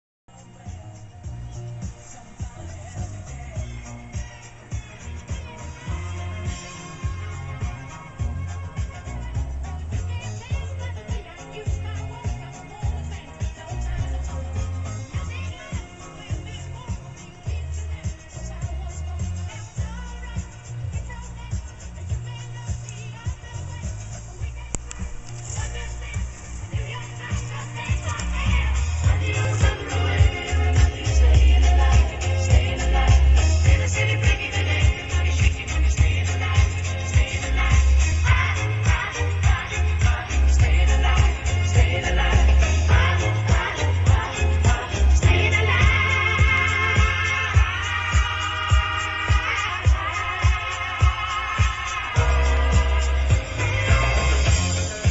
Updated Lights and Disco Ball in my Astro
Update from my last video. Now my diesel Astro has a Disco ball, laser projector, RGB strip lights and lots of TVs. Look at my other videos to see the videos of the diesel conversion.